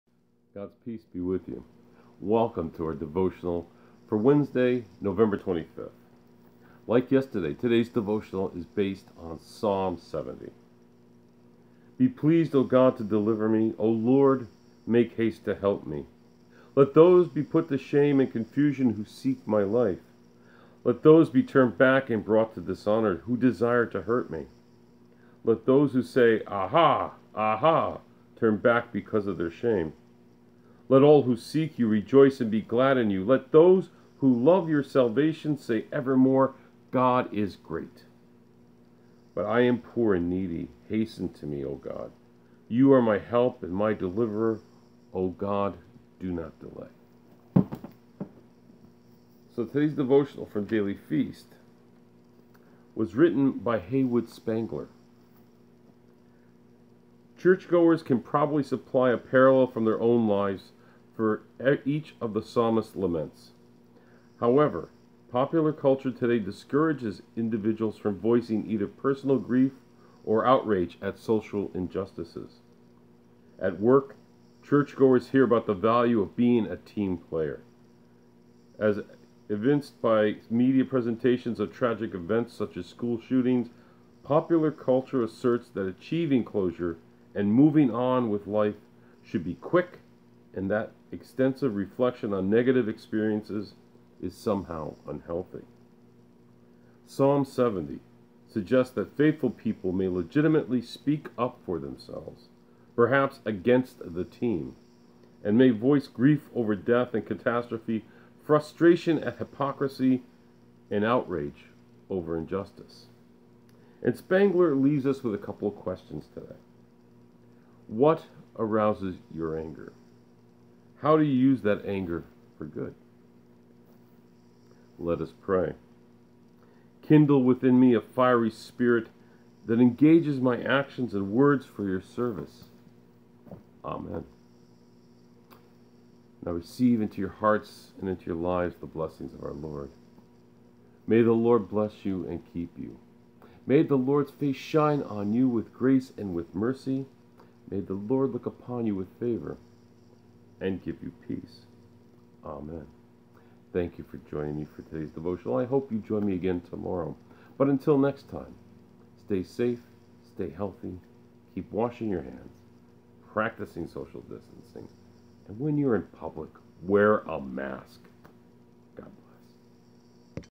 November 25 devotional for pandemic times
Devotional on Psalm 70 from Daily Feast and presented by Rev. Chris Krejlgaard